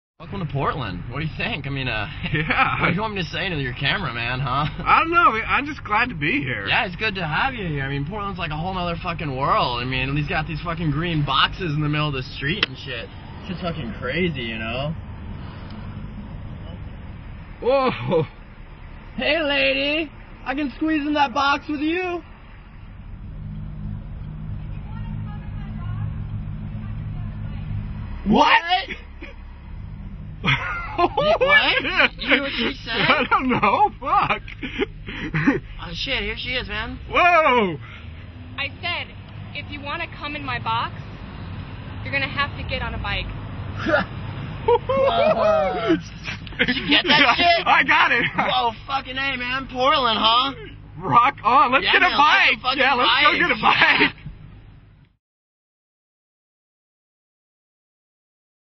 Get Behind the Bike Box
This video is not intended to be viewed by children and the language is foul.

PORTLAND, OR - There have been a lot of bike box videos uploaded to The internets latley. But how are bike boxes being recieved by your average Joes?

Well, I got my hands on some footage a couple jokers shot right down on Hawthorne Blvd. by the liquor store. I think I know who the biker chick is but I won't name names.

Anyway, this once again proves that Portland is much more fun because of bicyclists.

My advice to the bike box curious: get a bike and get busy in the bike box.